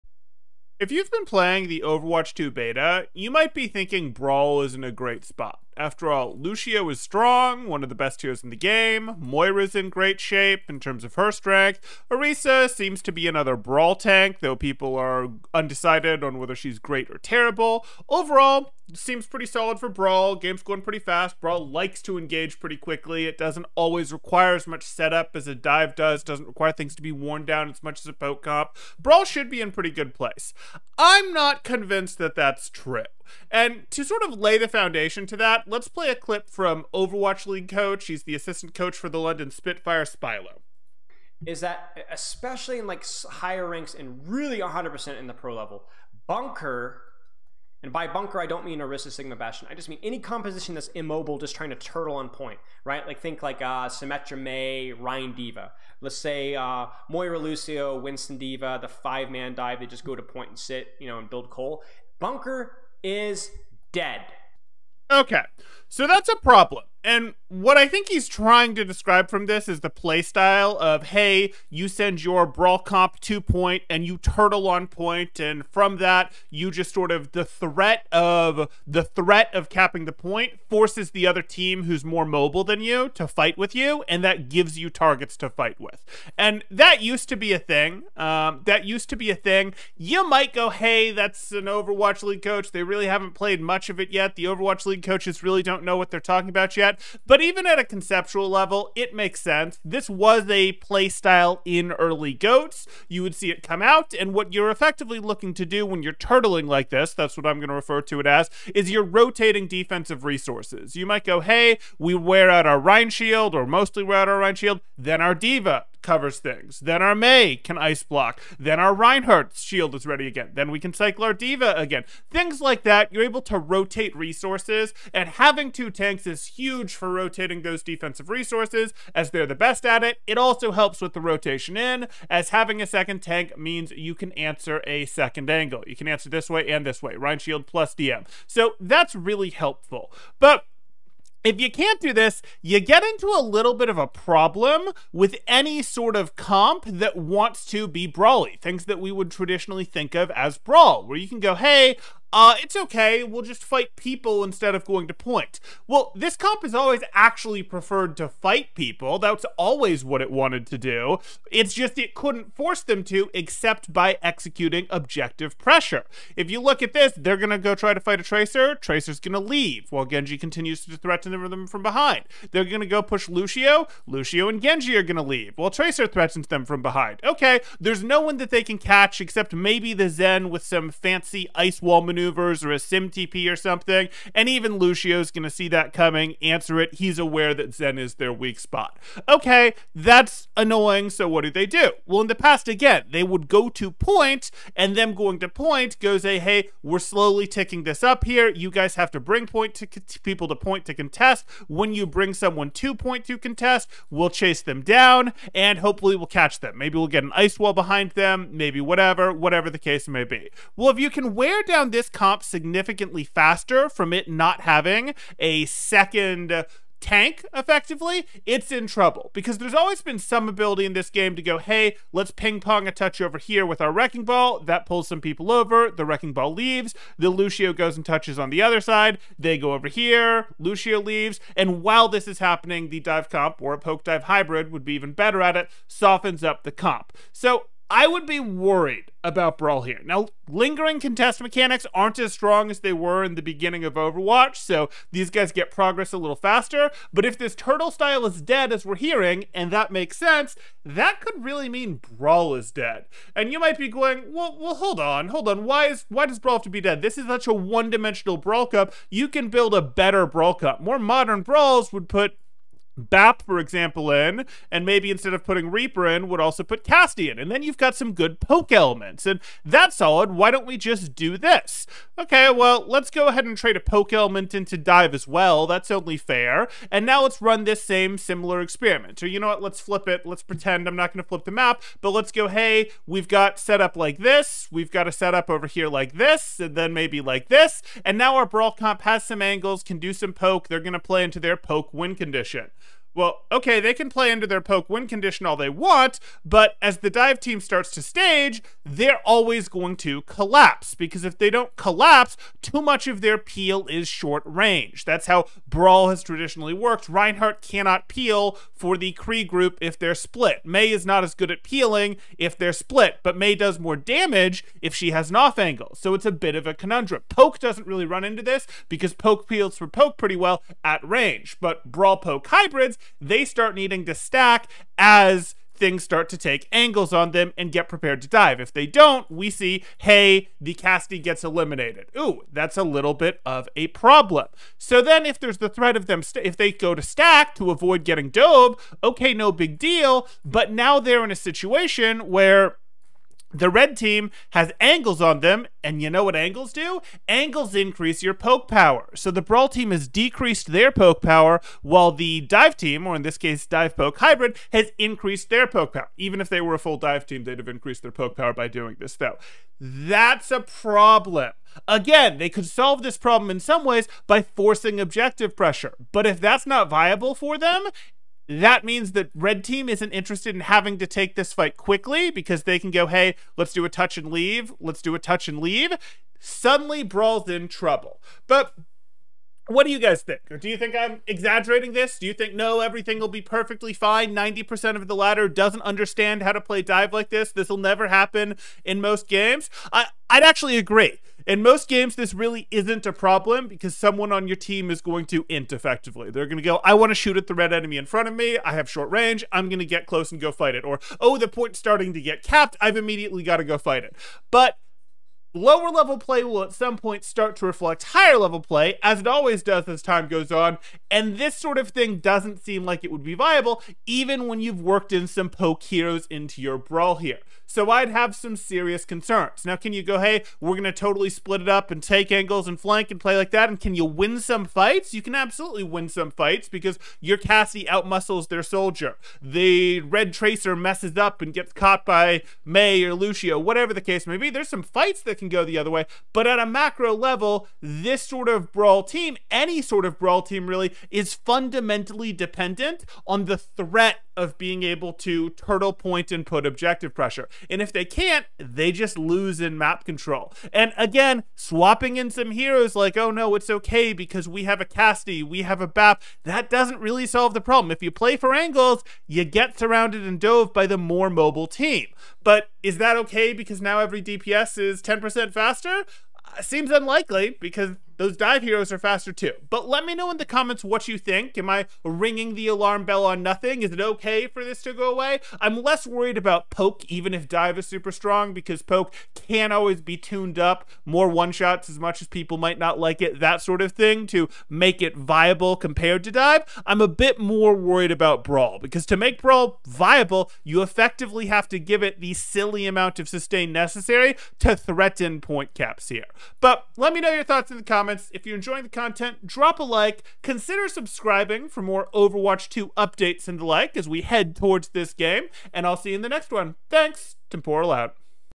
Did Blizzard Just Kill The Fan Favorite?
Strong Brawl Heroes!: (0:00)
OWL Coach Spilo & The Death Of Turtling: (0:30)
2nd Tank Take Away Defensive Rotation and 2nd Angle Protection: (1:30)
Brawl Needs THREAT Of Objective Pressure: (2:23)
Lingering Contest: (3:36)
Hybrids & Angles!: (4:13)
But Maybe I'll Be Fine?: (6:19)
Your Thoughts?: (7:50)